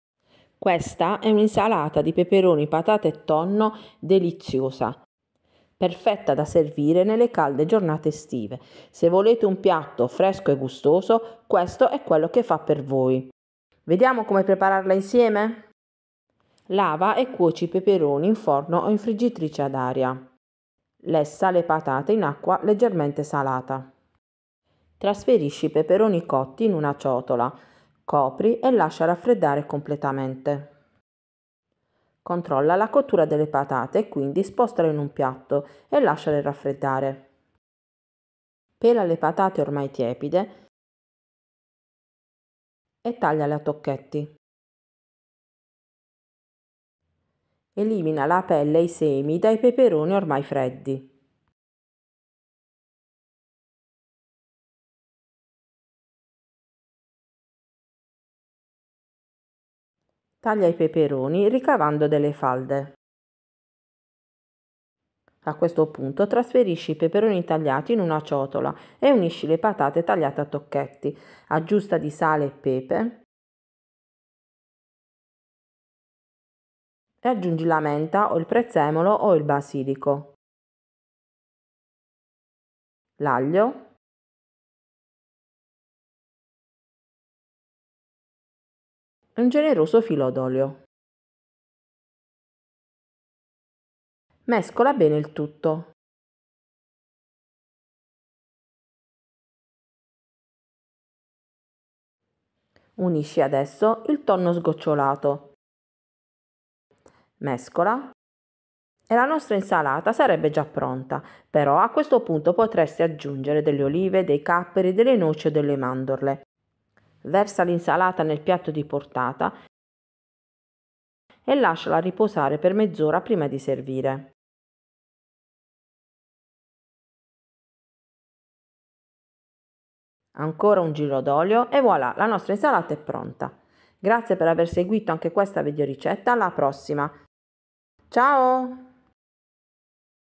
INSALATA DI PEPERONI PATATE E TONNO
L’Insalata di peperoni patate e tonno è un secondo piatto o  piatto unico freddo   tipicamente estivo , perfetto da servire sia a pranzo che a cena è anche da portare al mare o durante le gite fuori porta.

L’insalata di peperoni è una ricetta molto semplice e veloce da preparare ,  un piatto saporito e gustoso che sarà di sicuro gradimento: comoda , fresca,  versatile,  so prepara in anticipo ed è sempre pronta per essere servita in ogni occasione.
La ricetta può essere personalizzata con l’aggiunta di altri ingredienti , quelli base sono : peperoni,  patate e tonno , da condire con sale , pepe , olio,  menta o prezzemolo e aglio.

Alla ricetta base si possono aggiungere  :

olive,  capperi,  mozzarelle,  cipolle , pomodori,  cetrioli o barattieri .

Perfetta da gustare sia fredda che a temperatura ambiente ( io preferisco a temperatura ambiente) , l’insalata di peperoni  prevede due preparazioni principali:

la cottura dei peperoni ( in forno o in friggitrice ad aria )
e la bollitura delle patate .
In estate magari,  potreste procedere al mattino in modo da avere tutti gli ingredienti pronti per la preparazione.